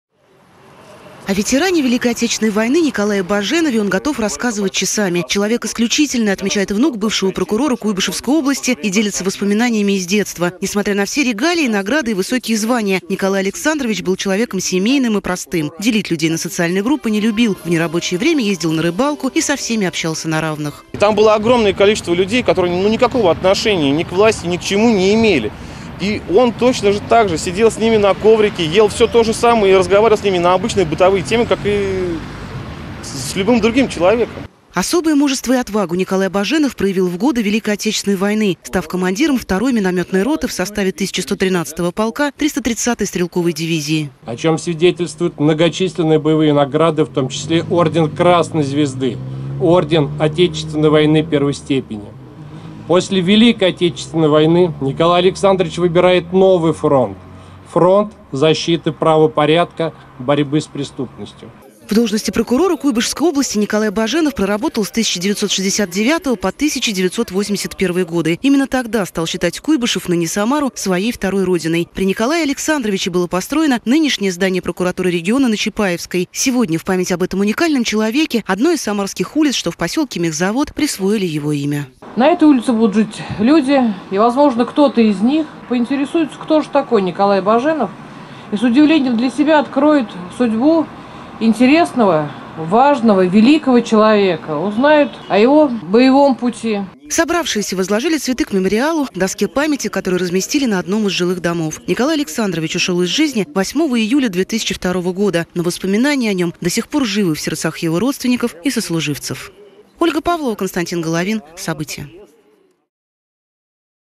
Улицу в Самаре назвали в честь знаменитого советского прокурора Николая Баженова
Памяти участника Великой Отечественной войны и бывшего прокурора Куйбышевской области. Одной из самарских улиц сегодня присвоено имя Николая Баженова. Первый заместитель генерального прокурора СССР принимал участие в ликвидации последствий на Чернобыльской АЭС и являлся обладателем множества наград. В торжественной церемонии возложения цветов к мемориалу, памятной доске, которую разместили на одном из домов по улице Баженова приняли участие глава Самары Елена Лапушкина, Прокурор Самарской области Сергей Берижицкий и члены ветеранских организаций. Тему продолжит Ольга Павлова.